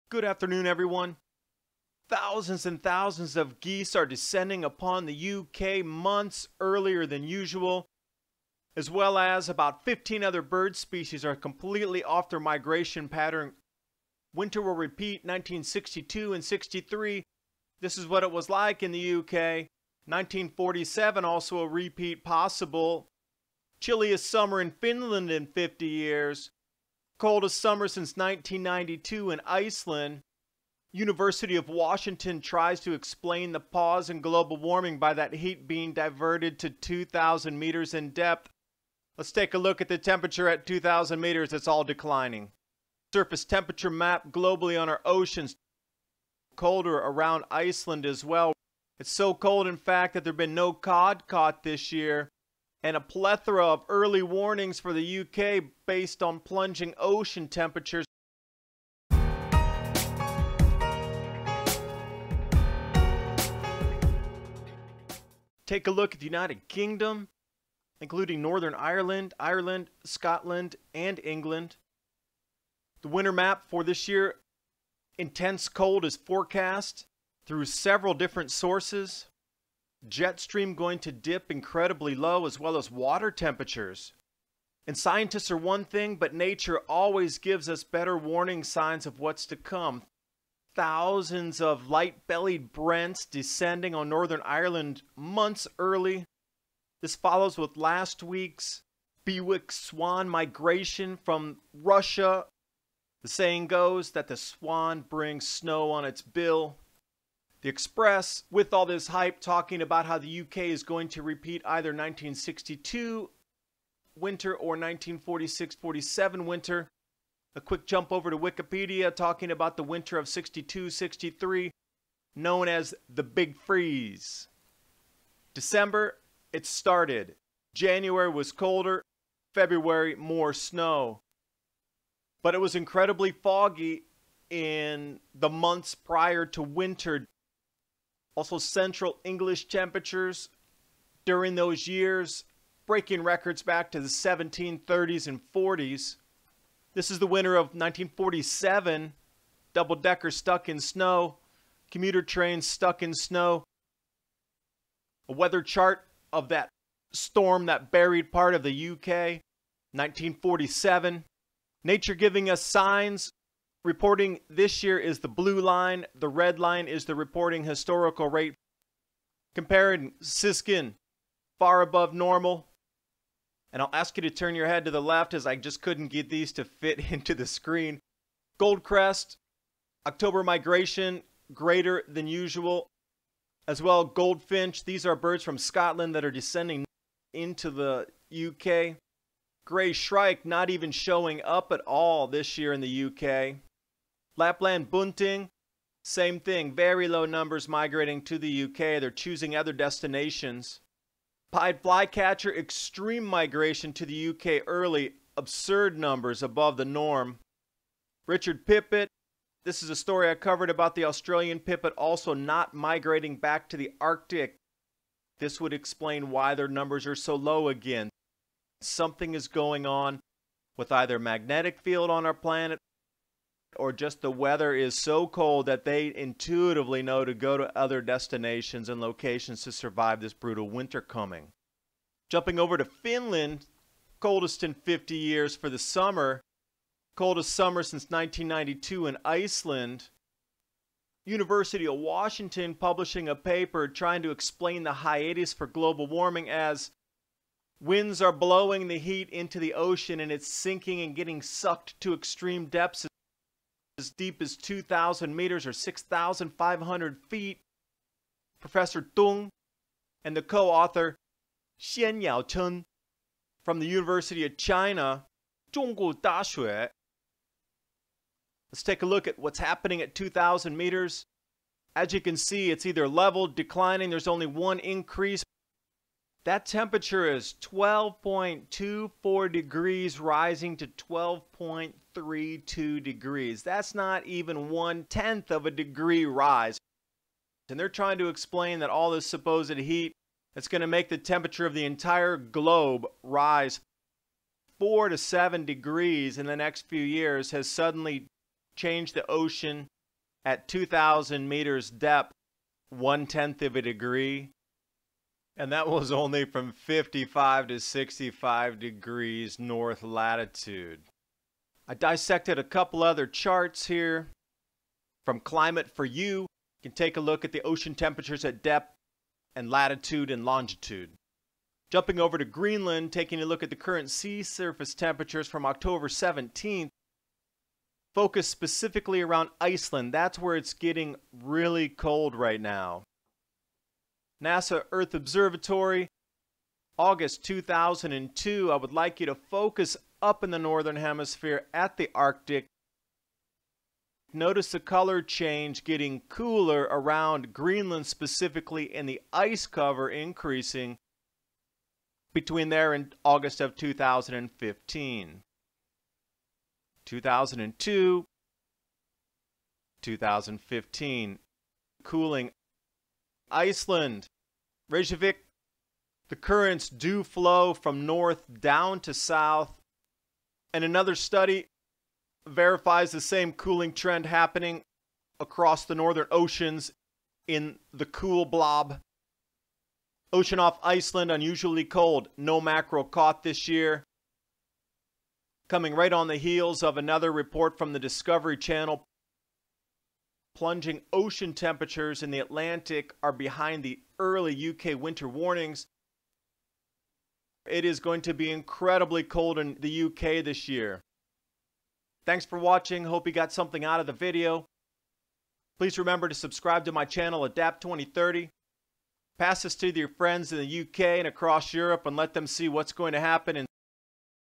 Migrating Birds & Scientists both Say U.K. Repeats Cold 1730 Winter | Mini Ice Age 2015-2035 (103)
Forecasts for the Uk and Europe show cold and snow. Cloder than normal waters will amplift this effect.